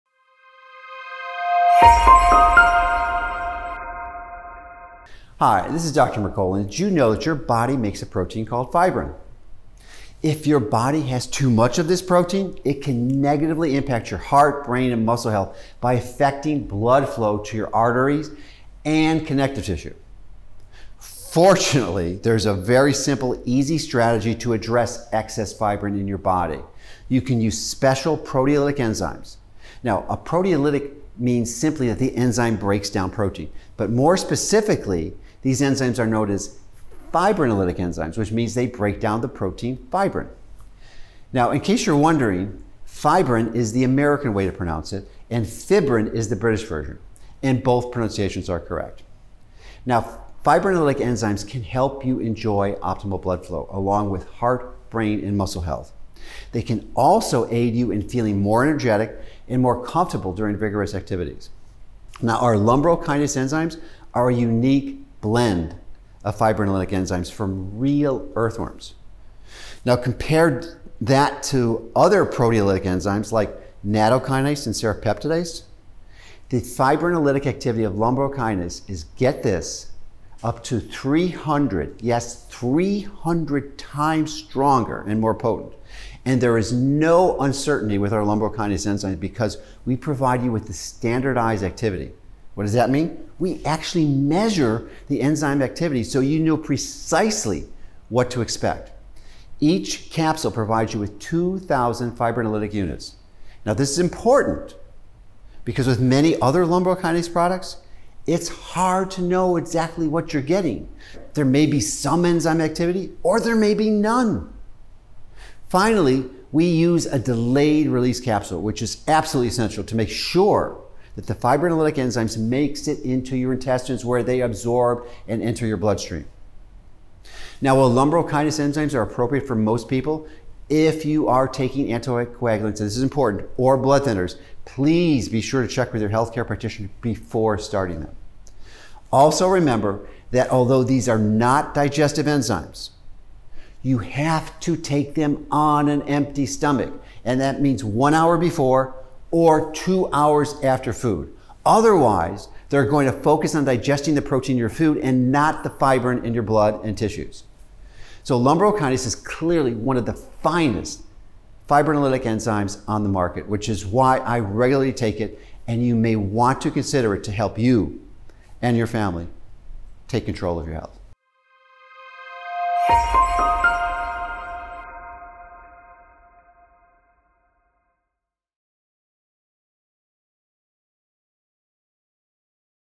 How LUMBROKINASE ENZYMES Support Heart Health and Blood Flow
Designed by animals, plants and bacteria to break down unwanted proteins, proteolytic – or fibrinolytic – enzymes help clean up your blood when taken between meals. Fibrinolytic enzymes, like lumbrokinase, also help remove fibrin, a protein that your body makes that can affect blood flow in your arteries and connective tissues.

Physician, New York Times best-selling author and founder of renowned natural health website mercola.com, Dr. Joseph Mercola talks about his LUMBROKINASE ENZYMES and how they can help you optimize your heart and brain health, as well support a healthy flow of blood throughout your body. Learn more about Dr. Mercola LUMBROKINASE ENZYMES or purchase your 30- or 90-day supply